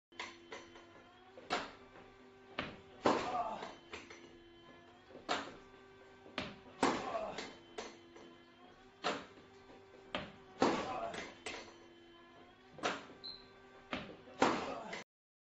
Tennis training at home with ball machine with net& Sony smart sensor
Cool to see your improvement by using this training combination!